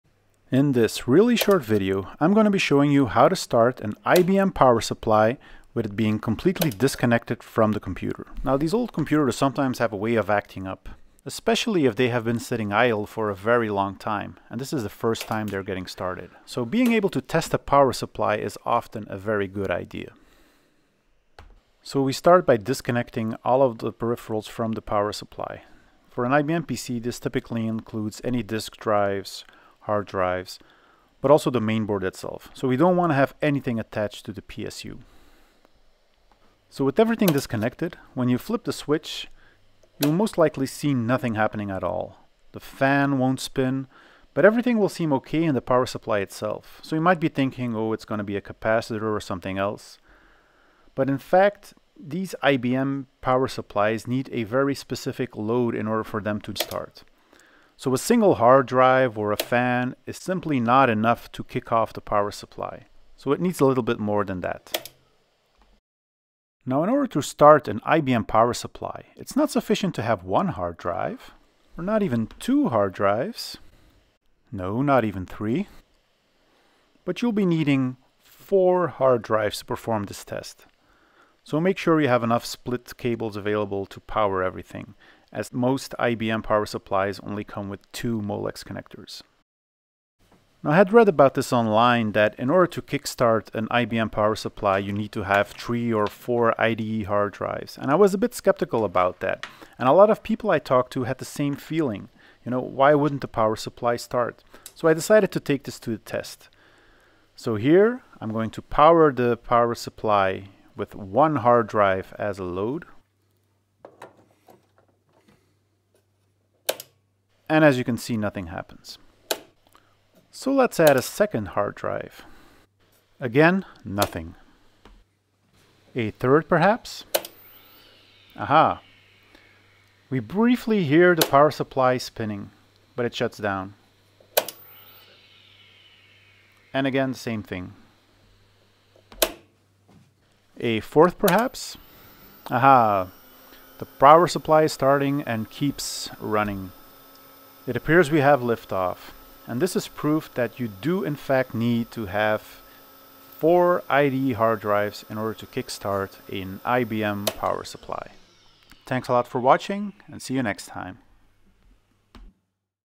IBM 5155 PSU testing with minimum load (free magic smoke and exploding caps included)
Testing an IBM power supply from an old IBM PC (51510 / 5160 / 5155)  without it being connected to the motherboard is not trivial.

I had read online that the IBM PSU won't start if there is insufficient load on it, and adding a hard drive or a fan is simply not enough.

I decided to put this to the test, and see what was needed in order to start the IBM PSU.